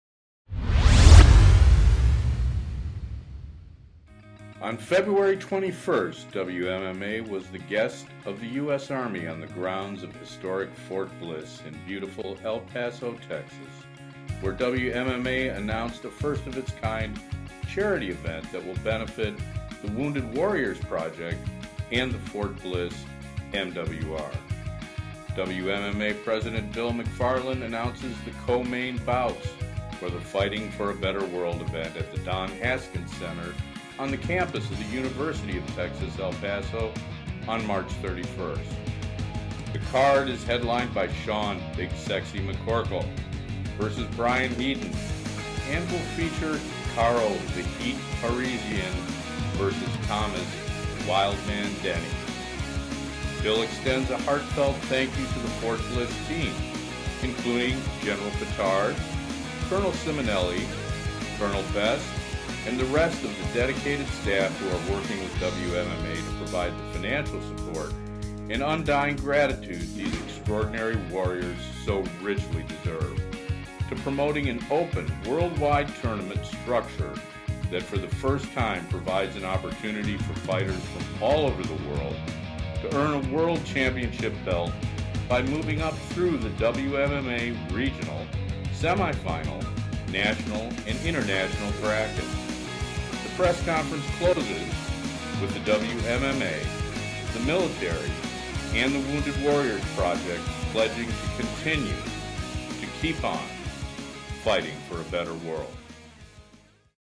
WMMA Sports Press Conference - Highlights
WMMA is hosted a press conference where it formally introduced the company and its mission to the world of mixed martial arts, the media and the public.

The press conference was held at Fort Bliss, TX on Tuesday, February 21, 2012 at 5 p.m. MST

WMMA Sports holds it's inaugural "Fighting For A Better World" charity event at the Don Haskins in El Paso TX 3/31/12 live on PPV. The event will be supporting wounded warriors by donating a large portion of revenue.

The card features 6'7 Sean "Big Sexy" McCorkle vs Brian Heden and former top contender Karo "the heat" Parisyan vs Thomas "Wildman Denny"

McCorkle vs. Heden: Fighting For A Better World

Main Card:
1. Sean McCorkle vs. Brian Heden (Super Heavyweight)
2. Thomas Denny vs. Karo Parisyan (170lbs)
3. Lyle Beerbohm vs. Derrick Noble (170lbs)
4. Drew Fickett vs. Kevin Knabjian (155lbs)
5. Jamaine Facey vs. Willie Parks (170lbs)
6. Rodney Wallace vs. Derrick Mehmen (205lbs)

Under Card:
7. Darrill Schoonover vs. Paul Buentello (265lbs)
8. Frank Gomez vs. Chris Gruetzemacher (145lbs)
9. Deutsch Pu'u vs. Esteves Jones (265lbs)
10. Lionel Lanham vs. Brad Peterson (185lbs)